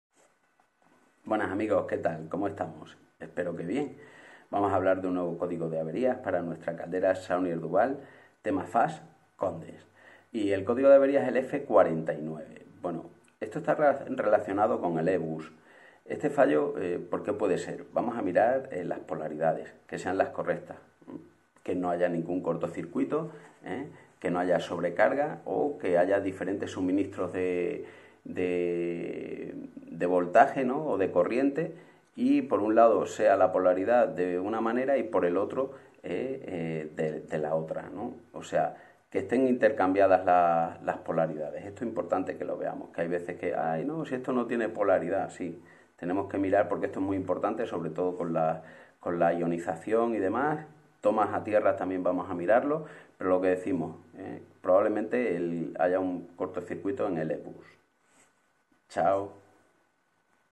Caldera Saunier Duval ThemaFast Condens codigo F49
En este vídeo vemos: Tutorial Caldera Saunier Duval ThemaFast Condens codigo F49

Herramientas

Soplete
Pinzas de presión
Alicates extensibles
Llave inglesa
Desatascador o chupona
Cinta métrica
Llave grifa

Fundidor
Barrena
Esterina
Productos
Abrazadera para tubería
Decapante